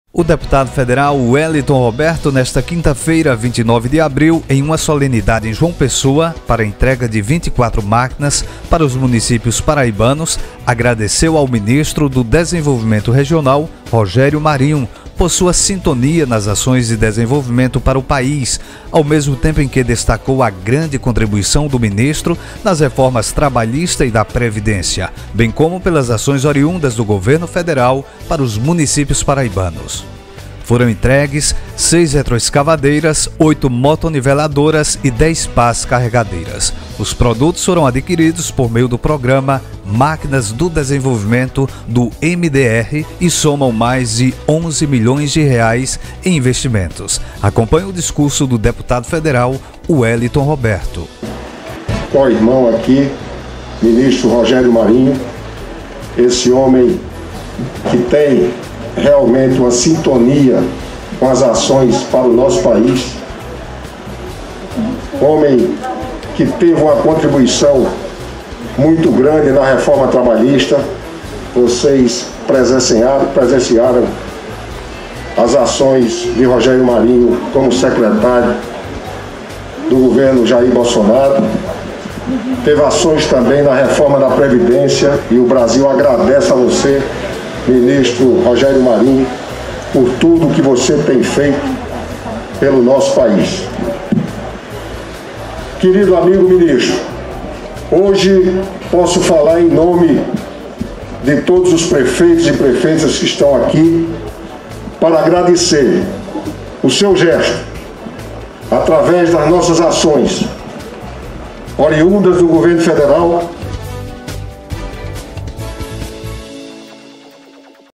Deputado Federal Wellington Roberto destaca ações do Governo Federal para municípios paraibanos
O Deputado Federal Wellington Roberto, nesta quinta-feira (29) em uma solenidade em João Pessoa para a entrega de 24 maquinas para os municípios paraibanos, agradeceu ao ministro do Desenvolvimento Regional, Rogério Marinho por sua sintonia nas ações de desenvolvimento para o país, ao mesmo tempo em que destacou a grande contribuição do ministro nas reformas trabalhista e da previdência, bem como pelas ações oriundas do governo federal para os municípios paraibanos.

Foram entregues seis retroescavadeiras, oito motoniveladoras e 10 pás-carregadeiras. Os produtos foram adquiridos por meio do programa Máquinas do Desenvolvimento, do MDR, e somam mais de R$ 11,6 milhões em investimentos.

Acompanhe o discurso do Deputado federal Wellington Roberto